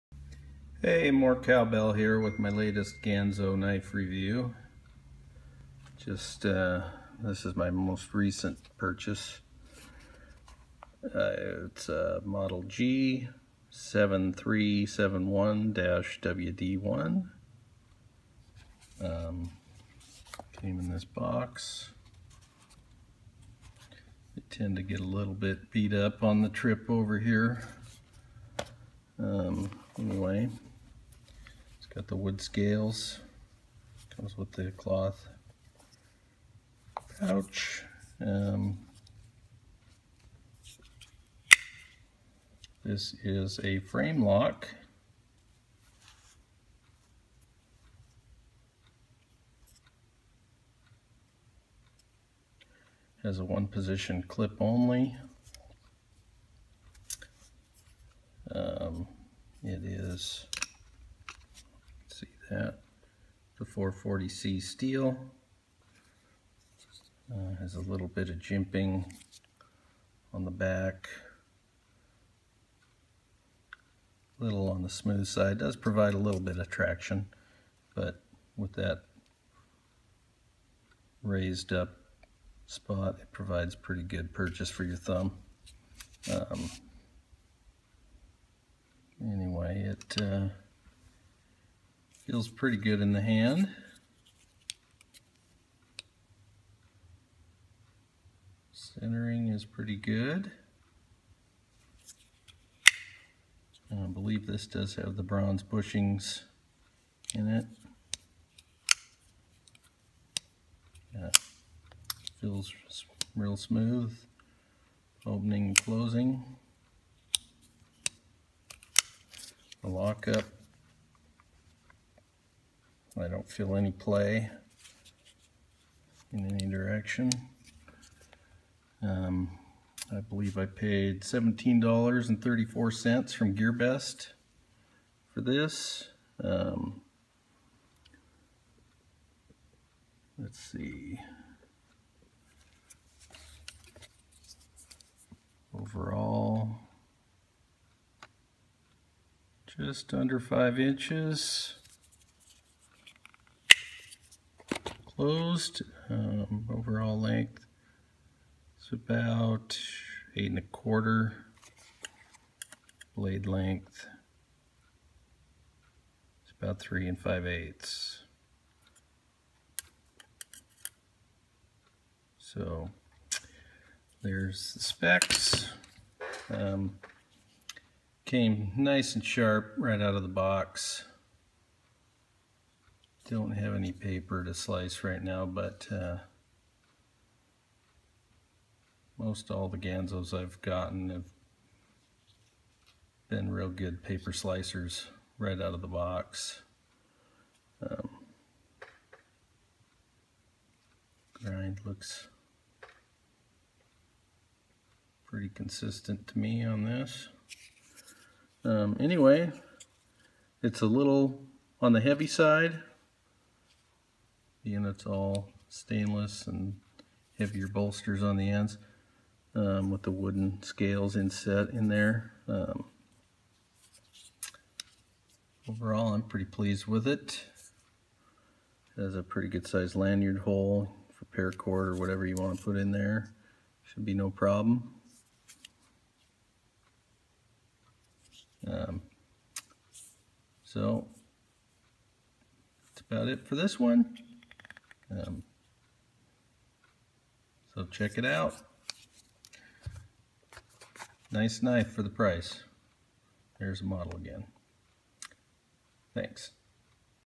Ganzo G7371-WD1
This is a Ganzo knife I purchased at Gearbest great knives for very little money